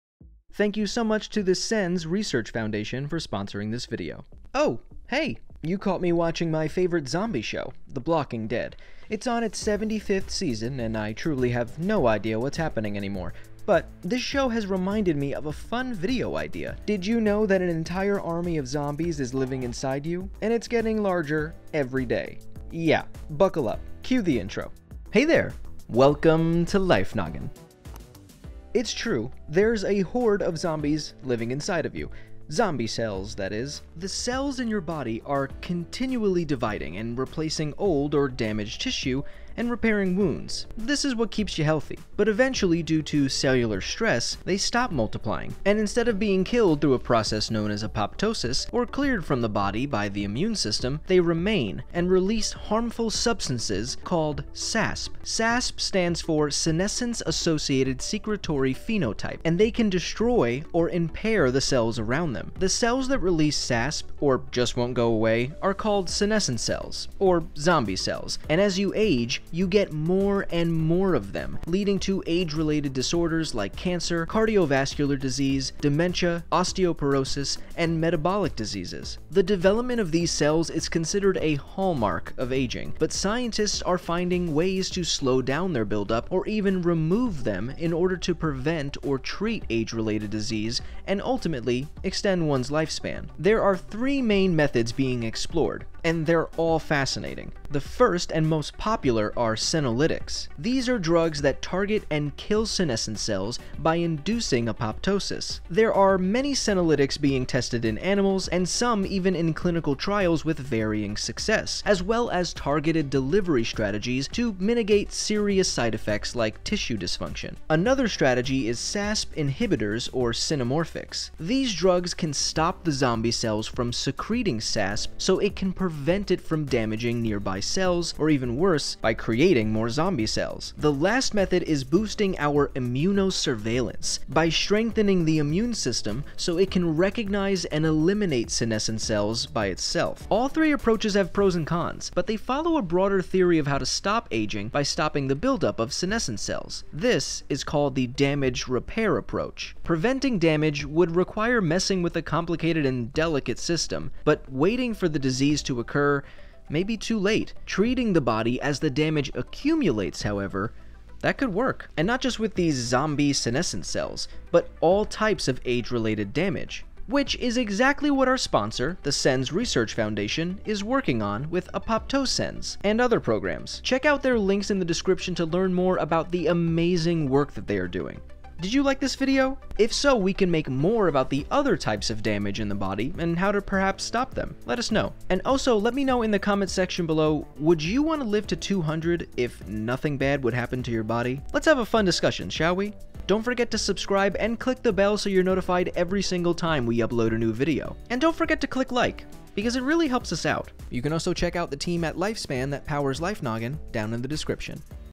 What’s the Zombie Cell Problem and How Do We Stop It?
How do we tackle the zombie cell problem happening to you right now?

Intro:
The cells in your body are continually dividing and replacing old or damaged tissue. This is what keeps you healthy. But eventually, due to cellular stress, they stop multiplying. And, instead of being killed and cleared from your body like typical dysfunctional cells through a process known as apoptosis, they remain and release harmful chemicals that destroy or impair the cells around them. These are called senescent cells and, as you age, you get more and more of them, leading to age-related disorders like cancer, cardiovascular disease, dementia, osteoporosis, and metabolic diseases. The development of these cells is considered a hallmark of aging. But scientists are finding ways to slow down or even reverse their accumulation in order to prevent or treat age-related diseases and ultimately extend one’s lifespan.

𝐅𝐨𝐥𝐥𝐨𝐰 𝐭𝐡𝐞 𝐋𝐢𝐟𝐞 𝐍𝐨𝐠𝐠𝐢𝐧 𝐓𝐞𝐚𝐦!
Produced by Maria Entraigues Abramson